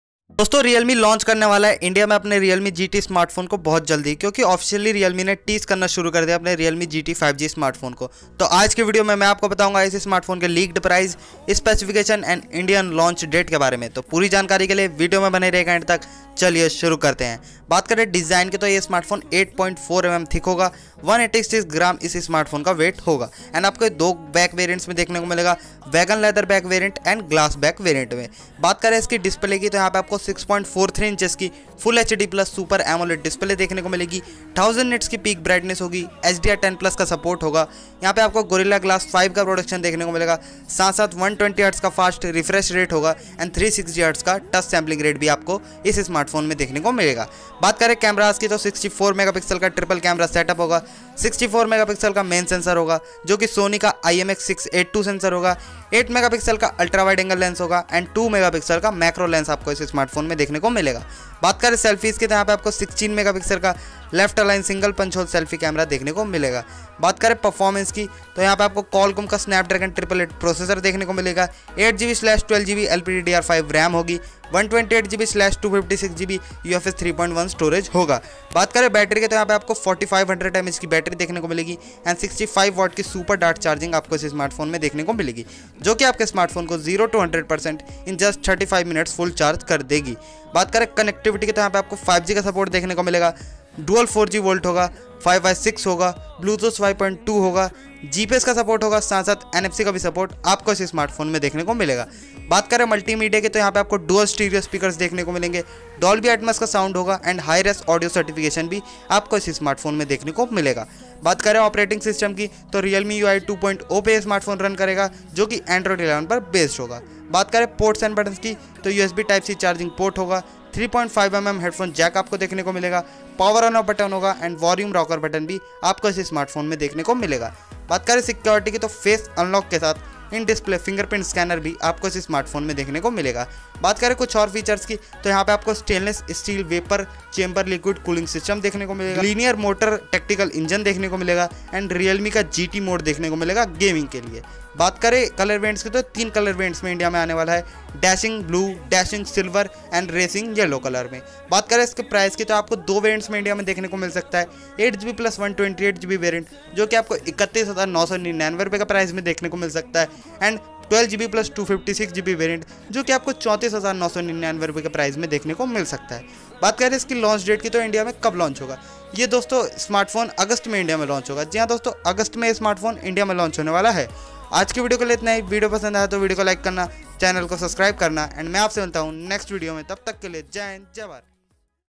REALME GT Launch Date & Price In India| Snapdragon 888,120Hz sAMOLED, 64MP IMX682, 65W Charging ₹30K
About Video:
In this video I am going to tell you the leaked price, specifications & Launch date of Realme GT Smartphone.
This smartphone is a flagship smartphone by Realme.